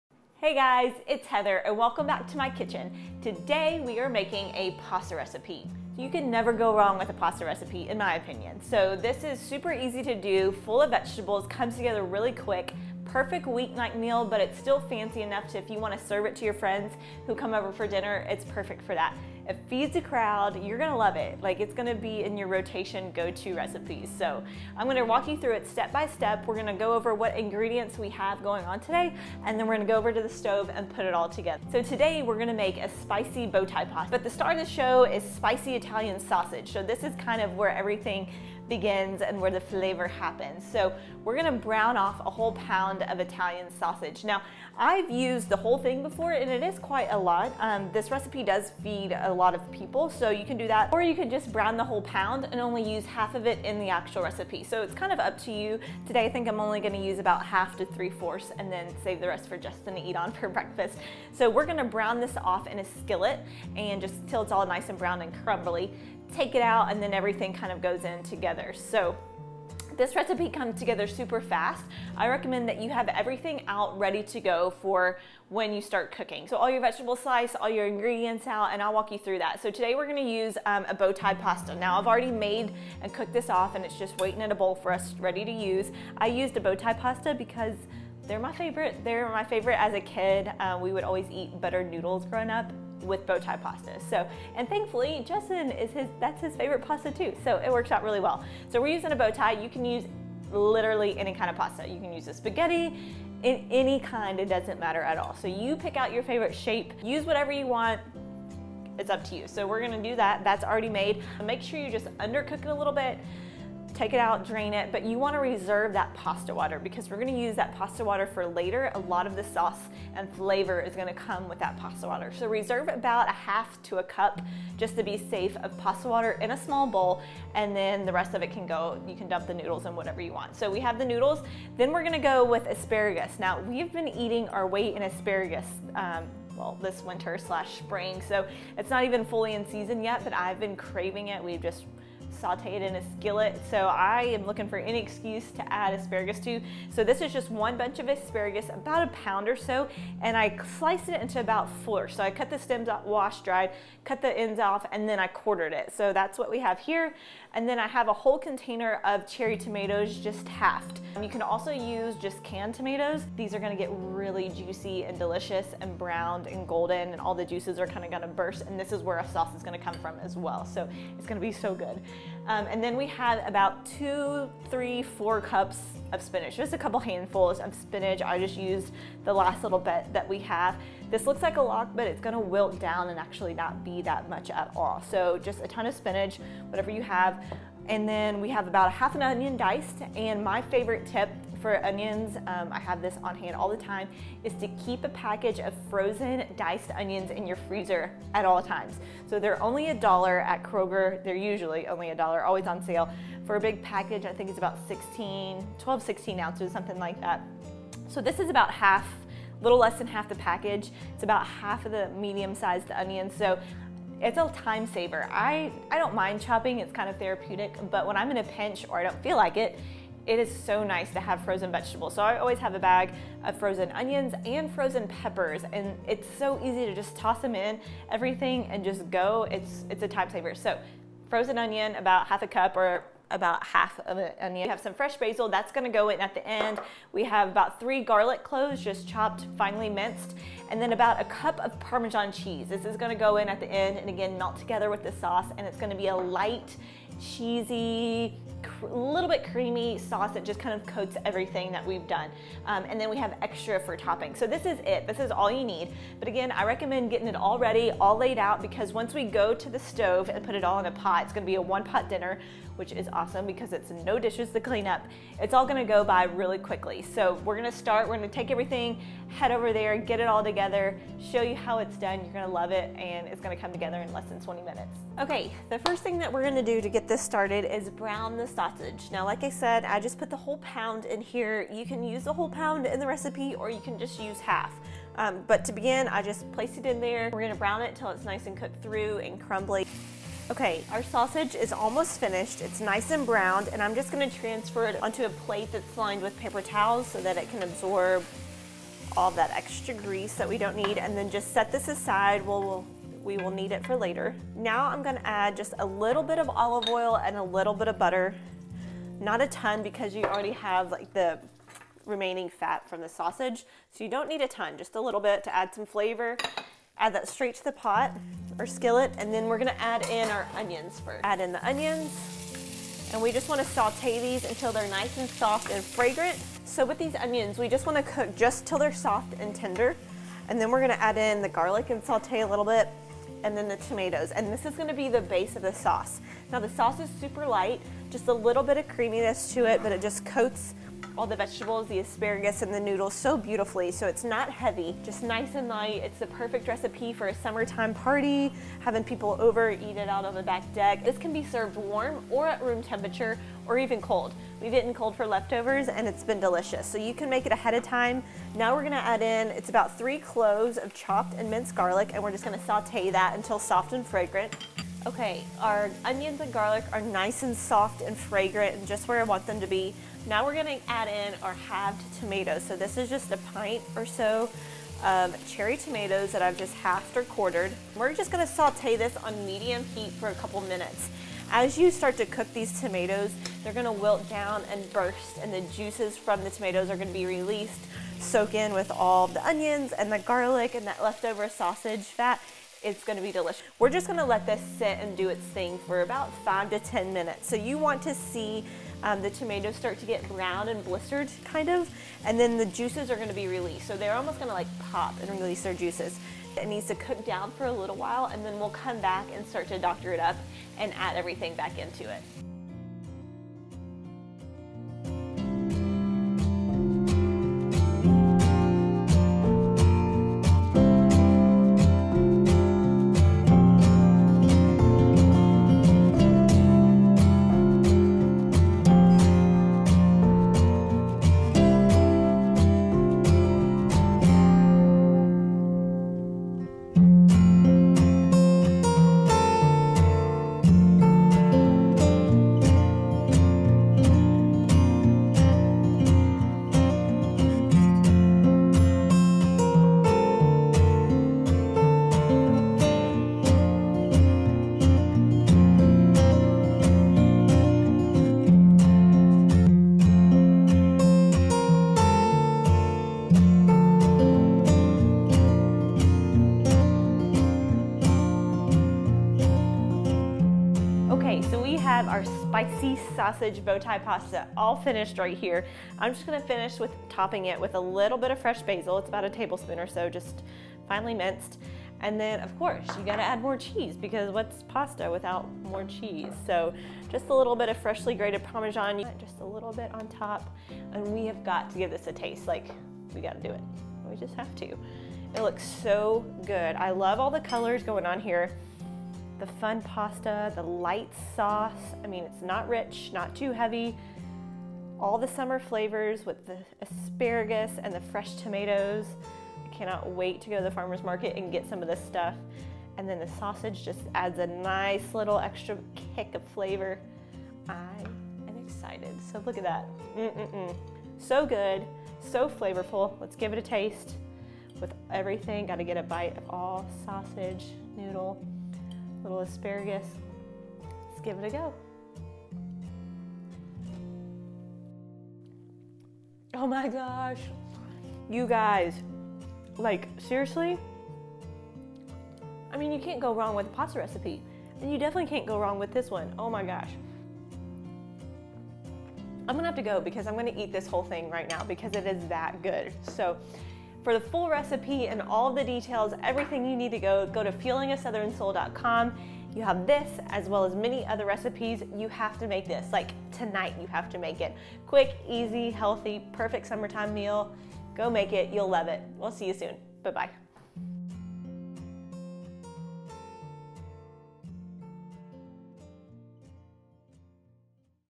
Spicy Sausage Bowtie Pasta | FUELING A SOUTHERN SOUL
1 pound Italian Sausage
16 Ounces Cherry Tomatoes
1 Bunch of Asparagus
2-3 Cups Spinach
1/2 Onion
1 Pound Pasta
3 Garlic Cloves
1 Cup Parmesan Cheese
2-4 Tablespoons Half & Half or Cream
1/2 - 1 Cup Pasta Water, reserved
1 Tablespoon Butter
1 Tablespoon EVOO
1-2 Tablespoons Fresh Basil

Place a pot of heavily salted water on the stove and bring to a boil. Add in pasta and cook for 5-7 minutes or until under al dente (you want them to be a little undercooked). Drain and reserve 1 cup of pasta water for later.
In a large cast iron skillet, add in sausage and cook until browned, making sure to break into bite-size pieces.
Remove the sausage and place on a plate lined with paper towels to cool.
In the same skillet add the butter & olive oil and onion and saute for 5 minutes until it starts to get tender. Then add in the minced garlic cloves and cook for another 2-3 minutes.
Add in the cherry tomatoes that have been halved and quartered and turn the heat to medium. Let cook for about 7-10 minutes until the tomatoes soften and begin to burst. Chop the asparagus into quarters and add to the skillet - cook for 3 minutes.
Add in 1 cup of pasta water and 2 tablespoons of half and half or cream. Stir together then add in cooked pasta and sausage. Toss together so everything is coated with the tomato sauce. Add the spinach then top with a lid to let wilt for 2-3 minutes. Once wilted, stir everything together and add the parmesan cheese. Add more cream or pasta water if more liquid is needed and cover to let cook for 5 more minutes. Add salt & pepper to taste and a pinch of red pepper flakes.
Remove from heat and top with fresh basil and more parmesan if desired. Serve immediately and enjoy!
Leftovers can be stored in the fridge for up to 3 days.